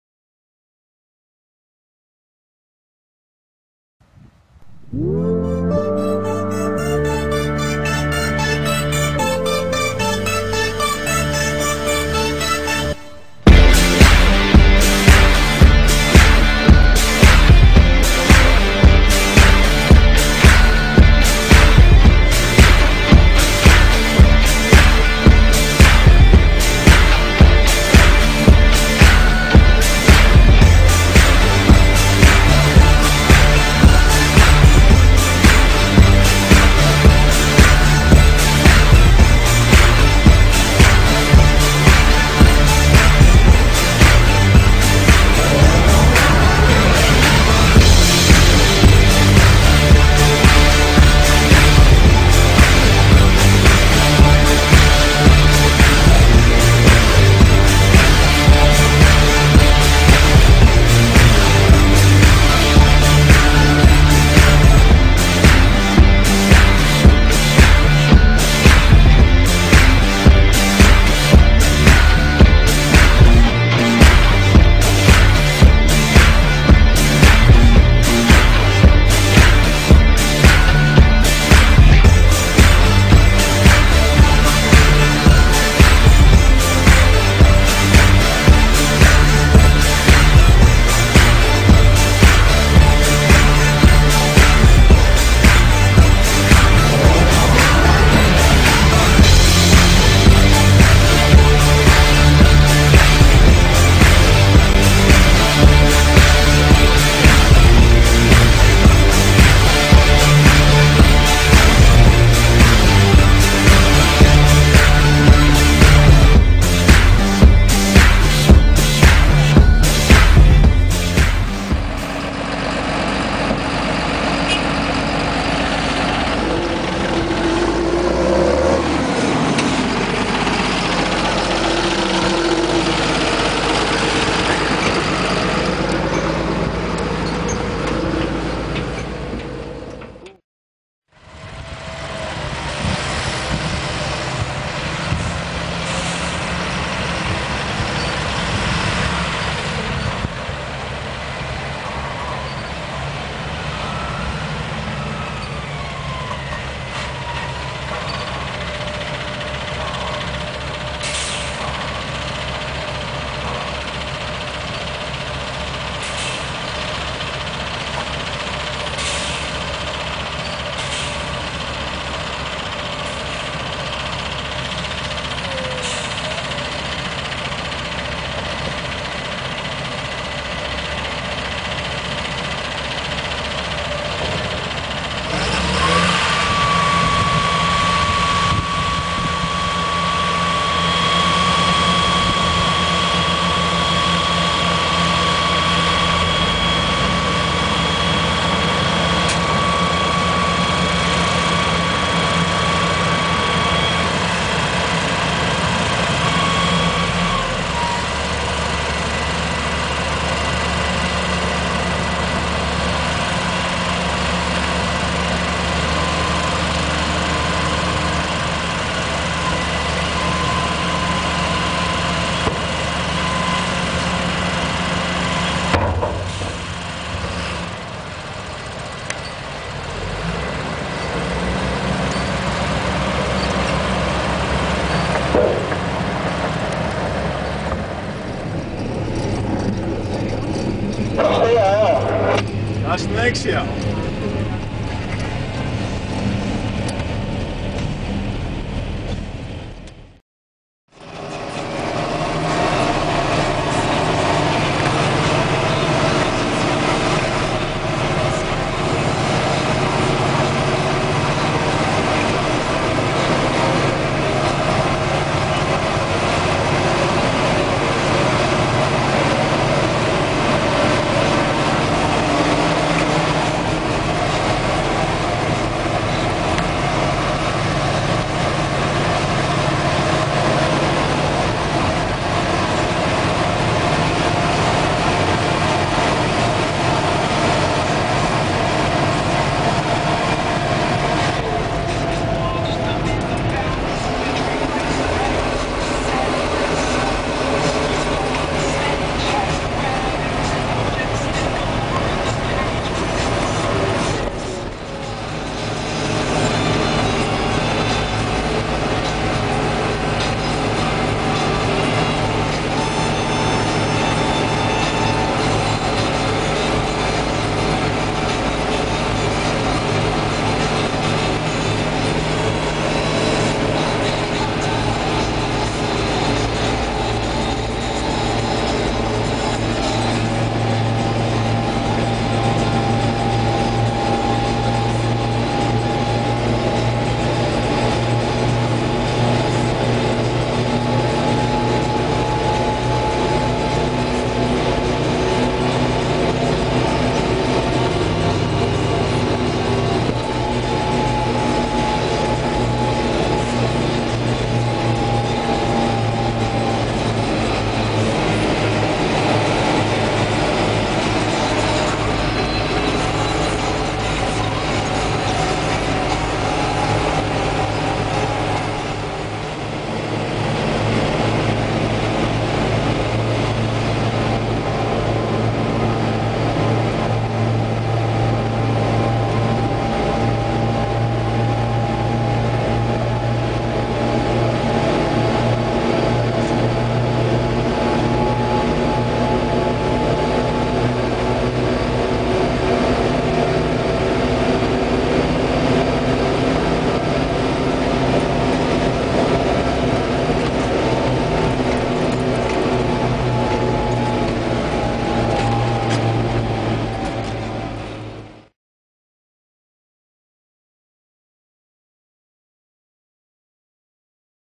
GECO-AGRI Grondwerken 2009
Grondwerken 2009
GECO-AGRI
Voort, Borgloon
John Deere 6920, 7720, 2x 7530, 7730
3x Beco 200, Beco 320